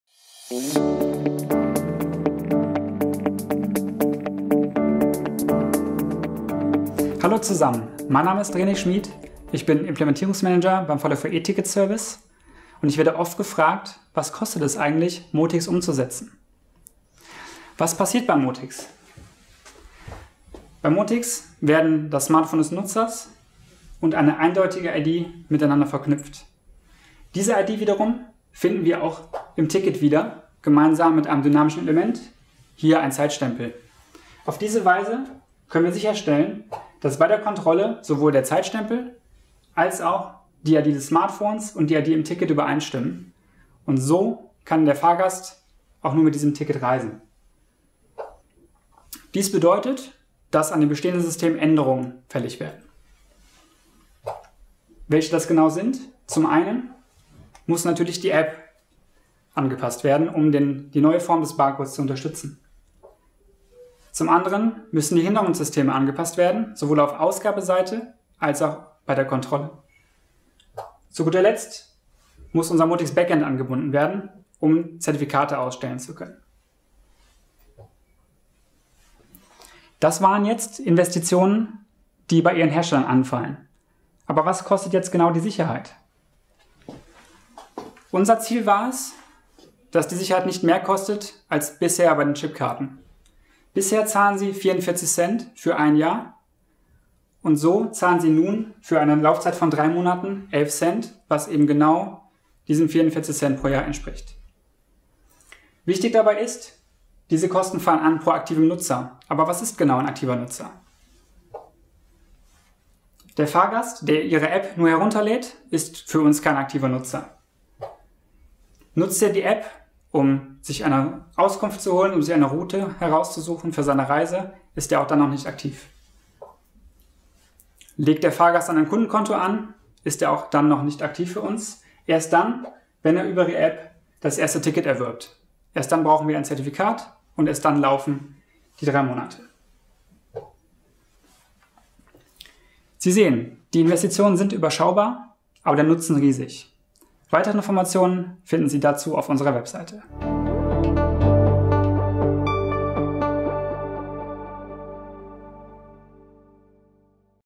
Was kostet der Motics die Verkehrsunternehmen?
Ob ein Zertifikat auf einer Chipkarte oder in einem Handyticket zum Einsatz kommt, macht finanziell kaum einen Unterschied. Unser Implementierungsmanager René Schmid erklärt in diesem Video,  was der Einsatz des Motics, unserem Kopierschutzes für Handytickets, kostet - und wann überhaupt Kosten anfallen.